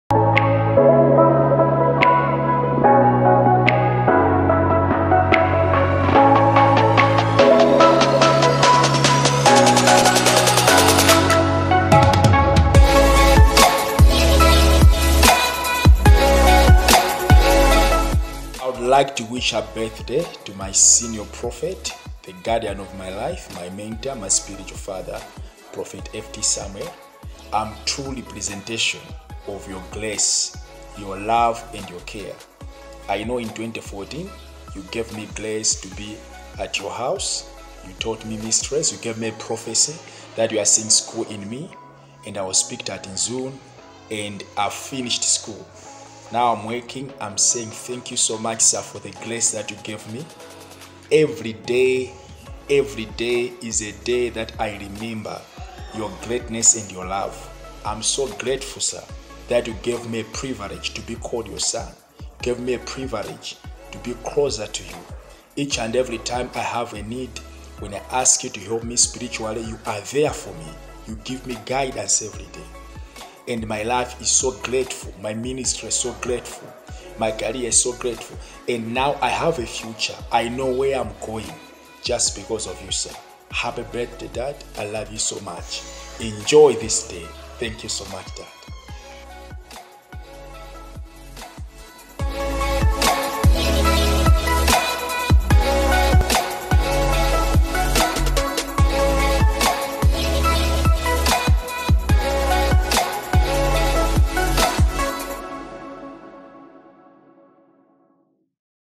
SPECIAL HAPPY BIRTHDAY MESSAGE FROM WONGANI NDHLOVI OF GRACIOUS SPIRIT MINISTRIES | LILONGWE
"I am what Iam because of you Prophet F.T. Samuel."